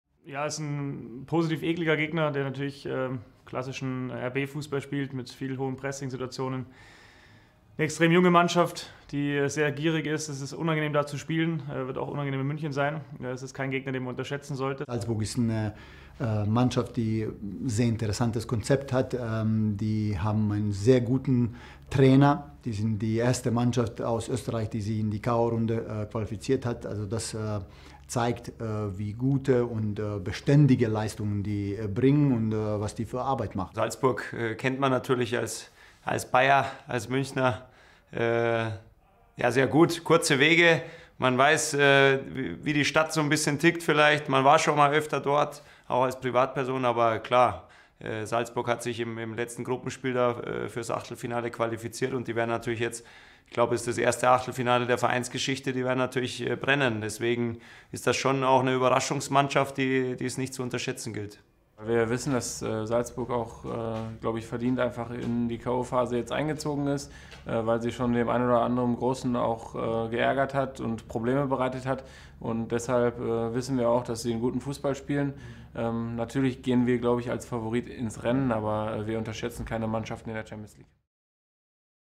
"Favorit" Bayern mit Respekt vor Salzburg: "Ein positiv-ekeliger Gegner" | SID
Die wiederholte Auslosung des Champions-League-Achtelfinals bescherte Bayern München mit RB Salzburg einen machbaren Gegner. Auch wenn die Bayern als klarer "Favorit ins Rennen gehen", werden Trainer Julian Nagelsmann und sein Team die Österreicher nicht unterschätzen.